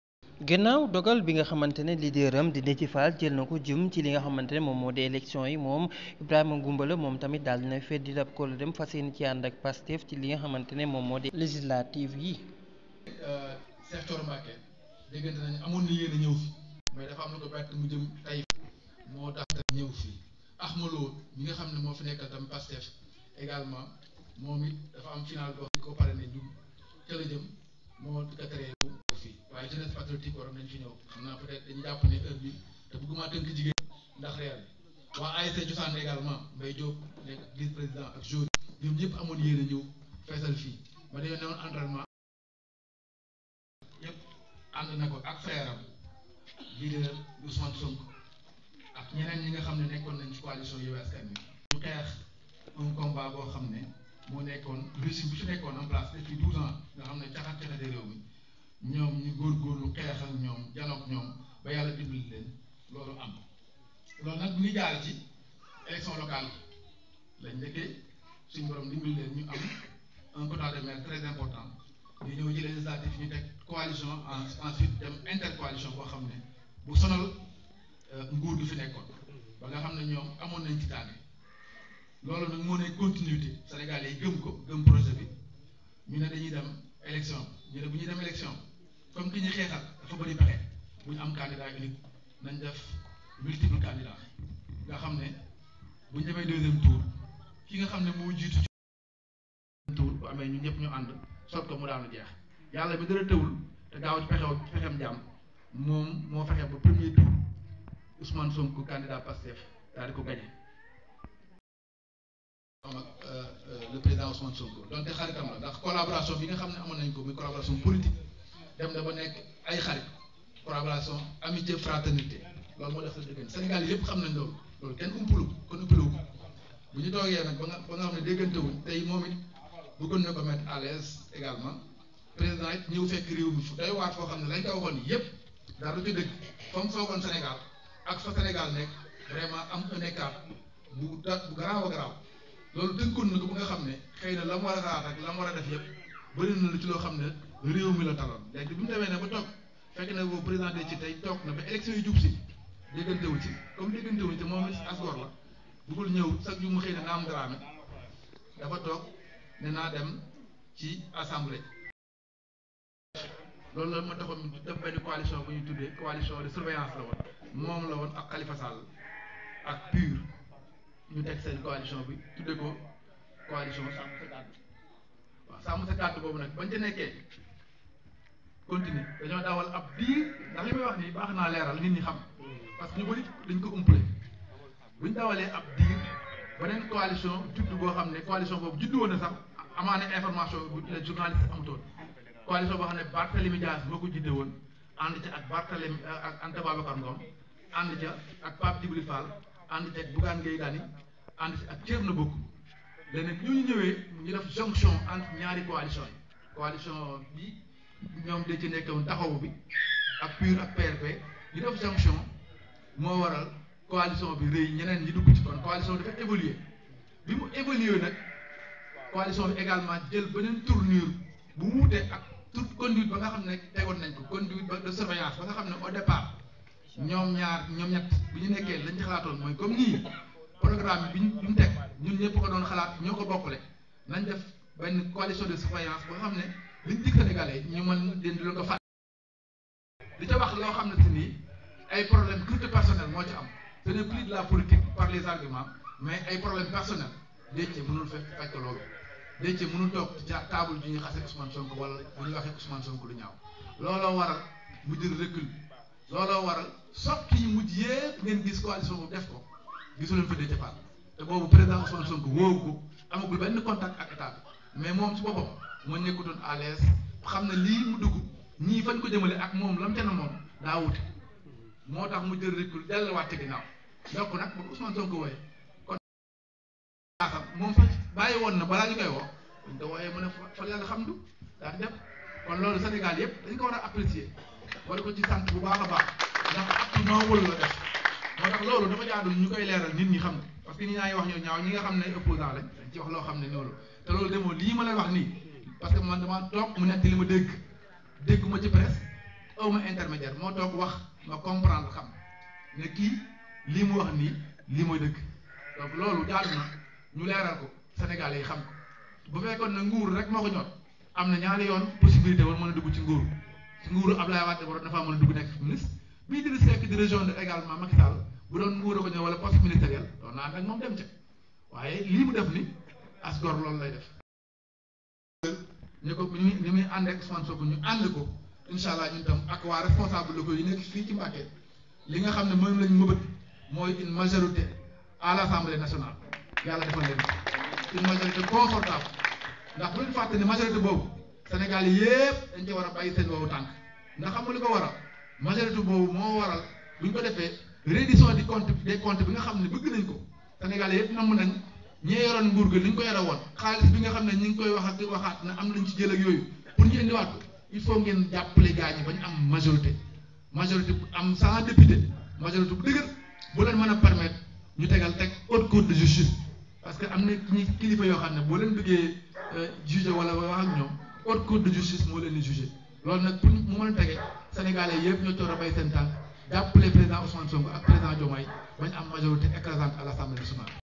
Prp Mbacke Dethié fall n'a pas trahi.. Ibrahima Goumbala rejoint le Pastef
Pastef reçoit un allié de taille...